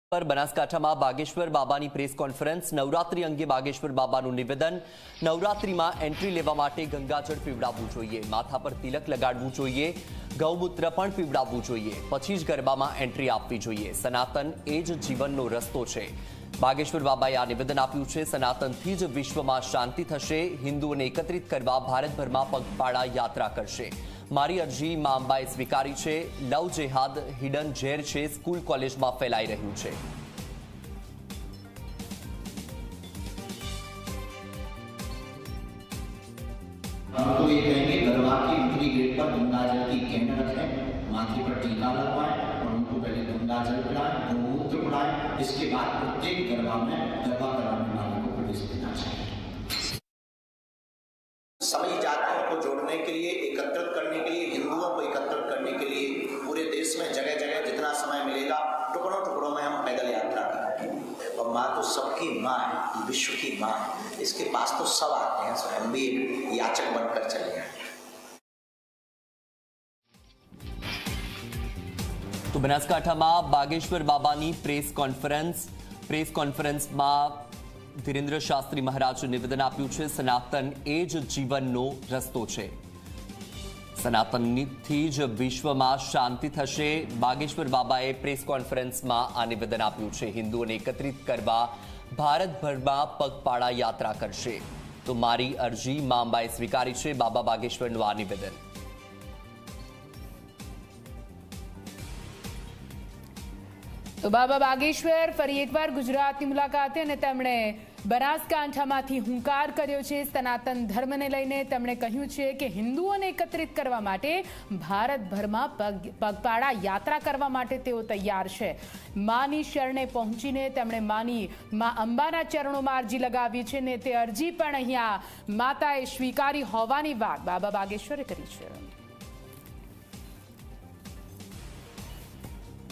Drinking Gangajal, tilak on head should be made compulsory to enter the Garba venues: Baba Bageshwar
ગરબાના સ્થળે પ્રવેશ મેળવવા ગંગાજળ પીવડાવવુ અને માથા પર તિલક ફરજીયાત થવું જોઈએ: બાબા બાગેશ્વર